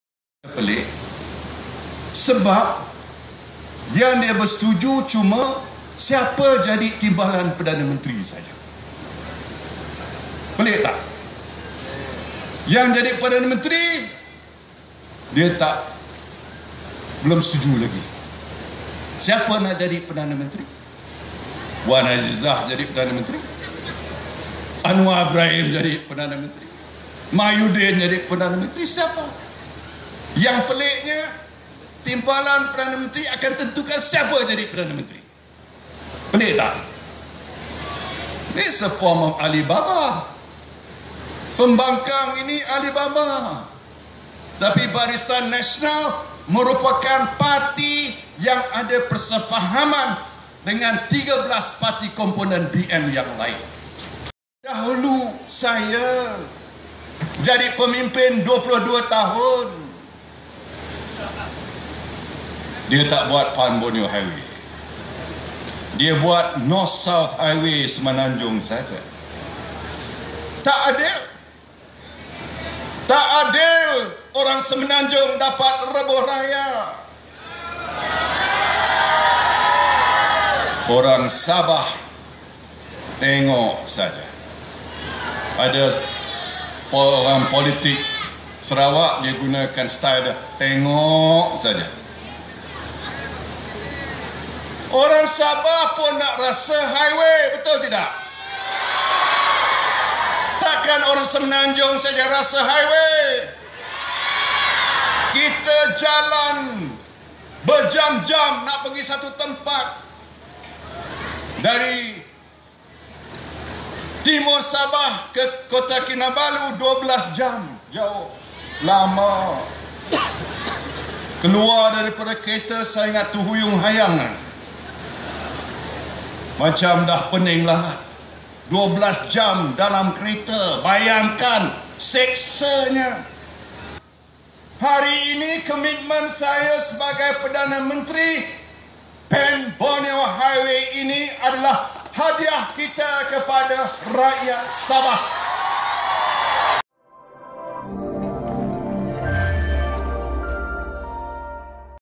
Opposition pact is like Ali Baba: Najib
SIPITANG: Prime Minister Datuk Seri Najib Razak described it as “peculiar” that the opposition has yet to reach consensus on who should become Prime Minister were they to come to power.